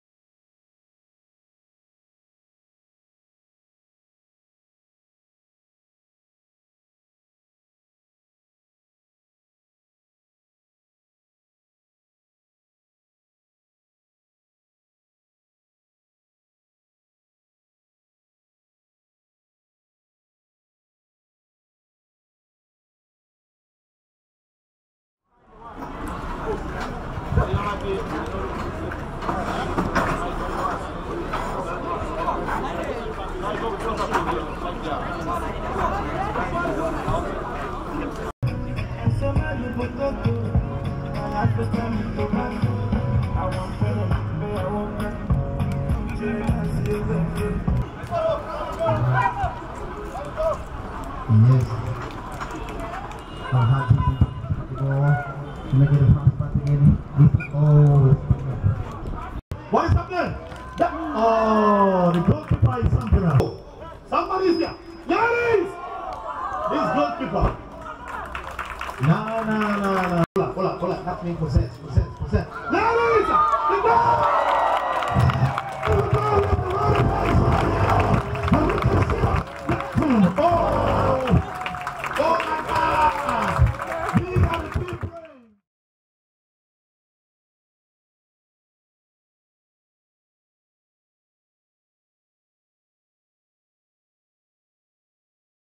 Sierra Leone Amputee Football Club
Sierra Leone’s civil war from 1991 to 2002 was the backdrop to the 2006 movie, Blood Diamond, with Leonardo DiCaprio. The brutality of the rebel force left thousands of young men with amputated limbs. Survivors formed the famous Flying Stars Amputee Football Club, shown here playing an exhibition game against a team assembled by the Silver Cloud cruise ship during its visit on April 15, 2018.